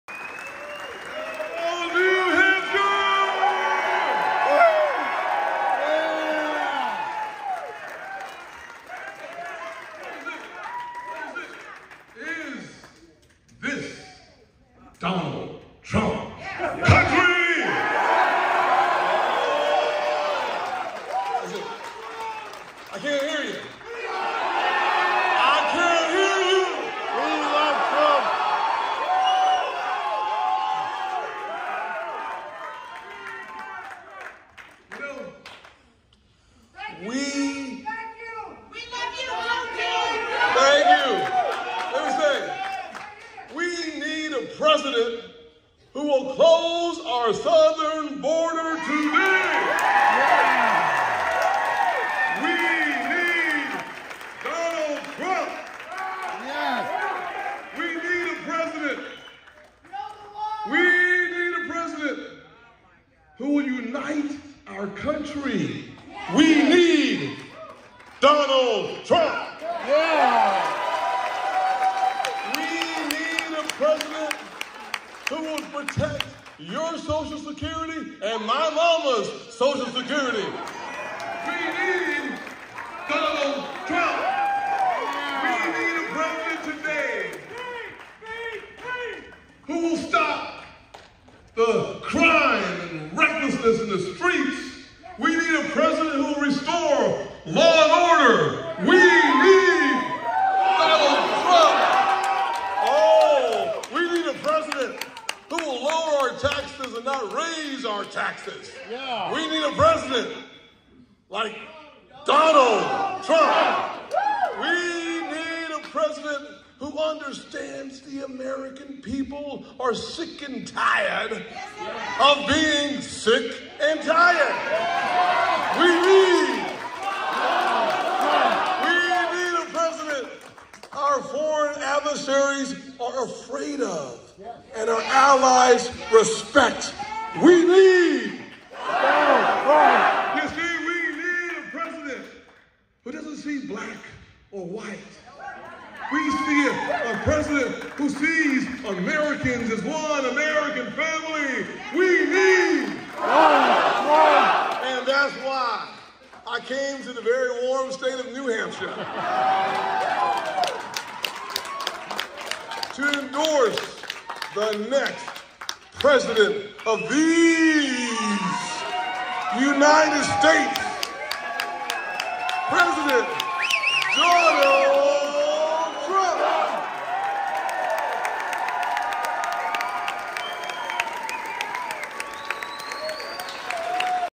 Sen. Tim Scott endorses Donald Trump ahead of New Hampshire primary
"Senator Tim Scott Endorses Donald Trump Ahead of New Hampshire Primary! 📢 Watch this exclusive video for in-depth coverage of the surprising endorsement that could reshape the political landscape. 🇺🇸 Stay informed on the latest political developments and reactions.